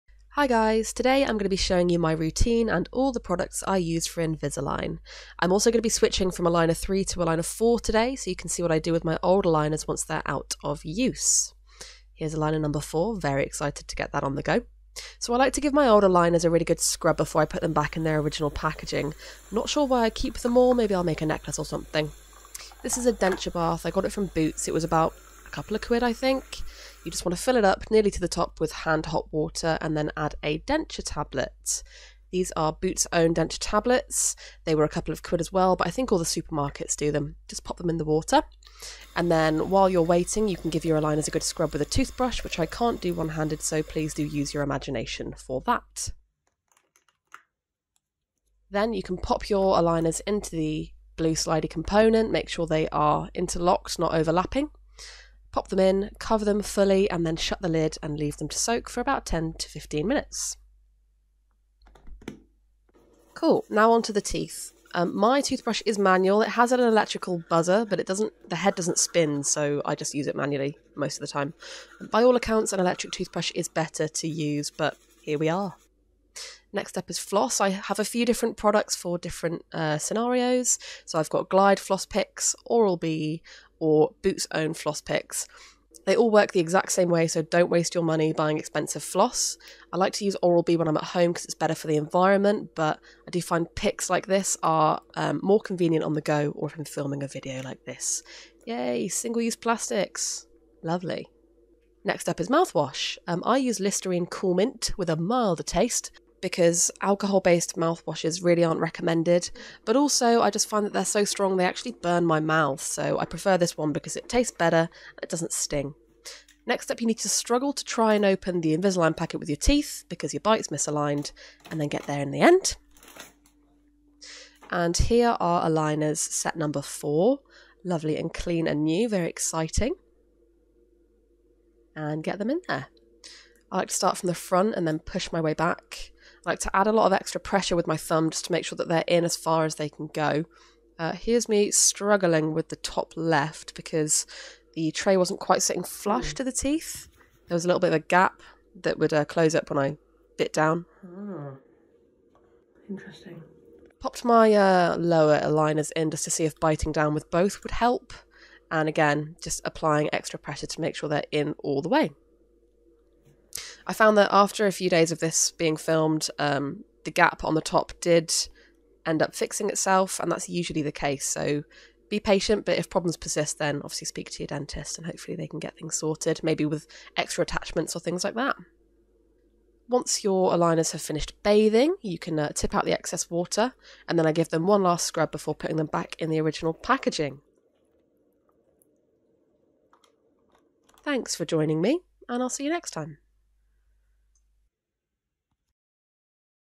Invisalign ✨ Routine & Products
Join me as I switch from Aligner #3 to Aligner #4!